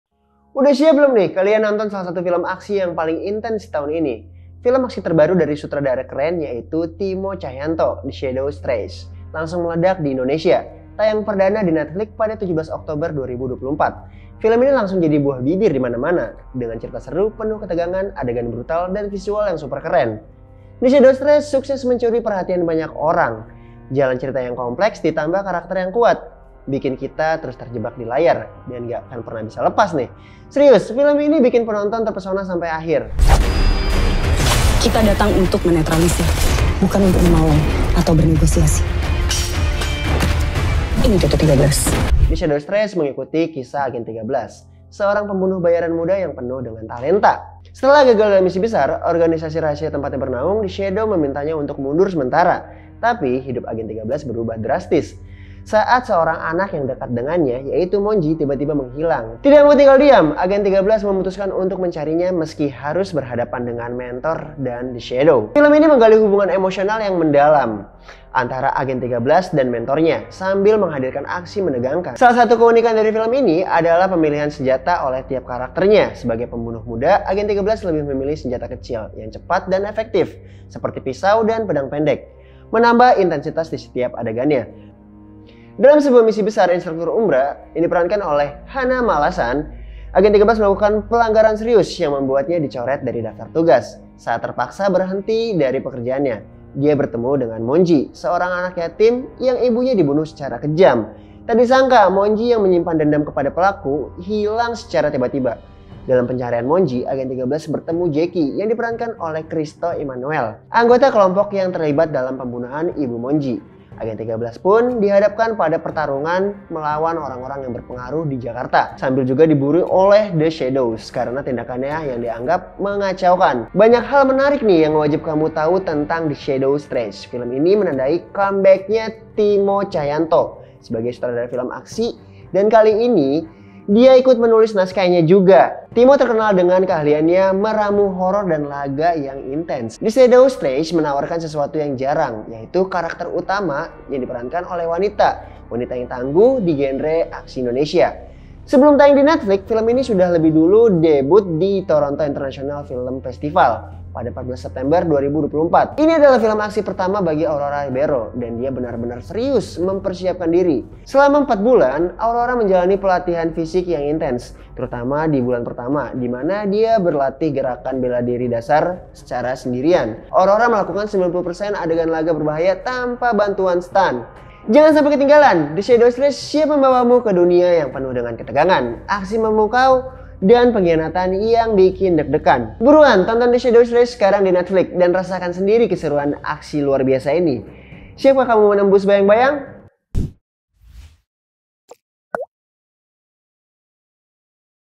Review Film The Shadow Strays | OBSESI
The Shadow Strays bercerita tentang seorang pembunuh belia yang terampil melawan mentornya untuk menyelamatkan seorang anak laki-laki dari sindikat kejahatan yang kejam. Ia siap menghancurkan siapa pun yang menghalanginya. Yuk simak reviewnya!

[Indonesia]